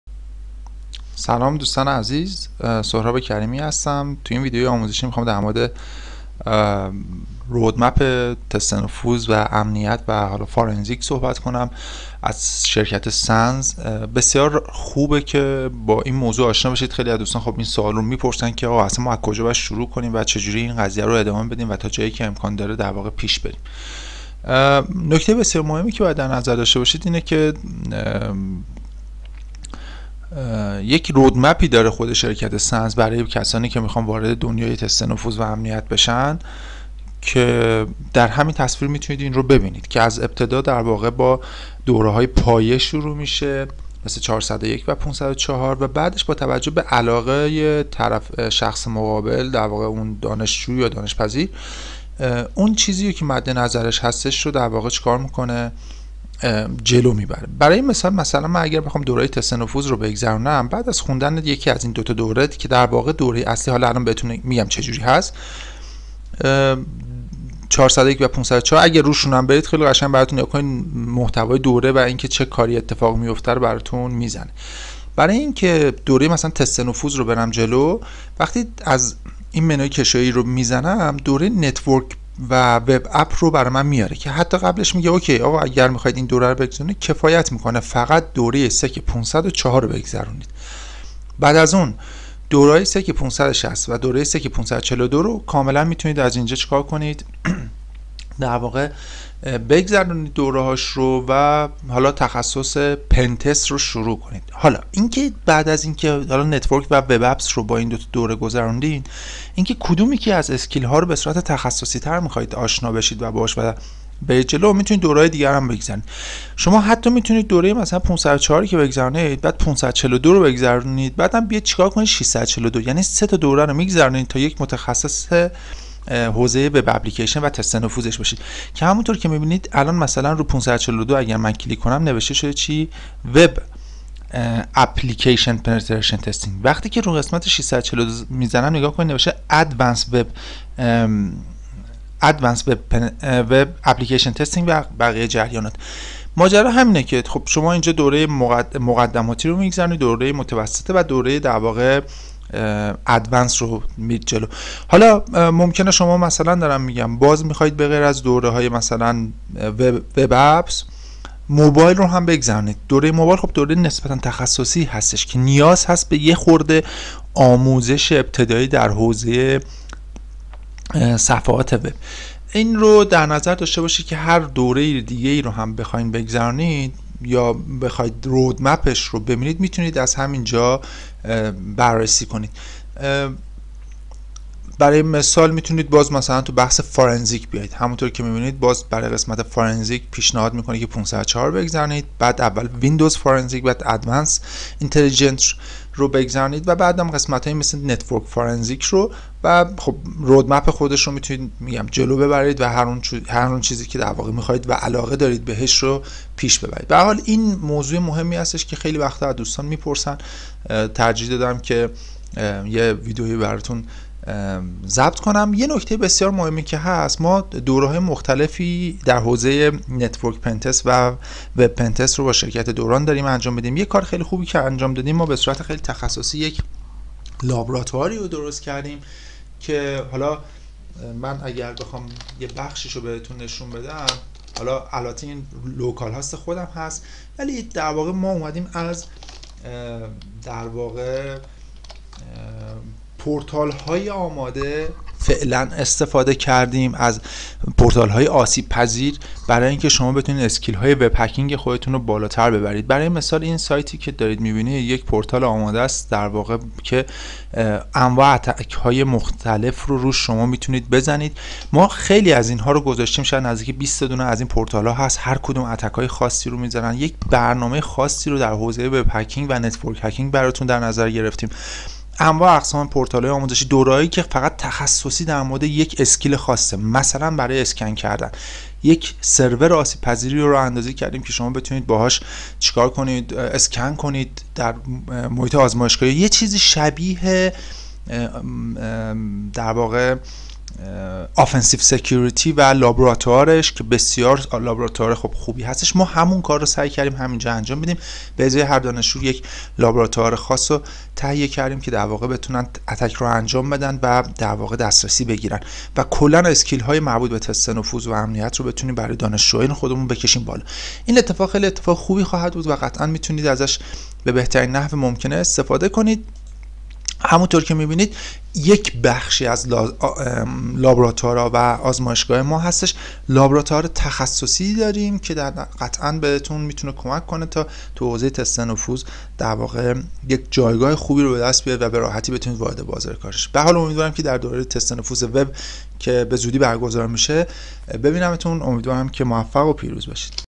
SANS معرفی مسیر آموزشی تست نفوذ و امنیت
.در این ویدیو مسیر آموزشی تست نفوذ و امنیت شرکت سنز معرفی شده است
و به سوالات زیر پاسخ می‌دهد:
از کجا شروع کنیم؟
چه دوره‌هایی را باید بگذرانیم؟
محتوای دوره‌‍‌ها و زیر مجموعه‌ها به چه صورت است؟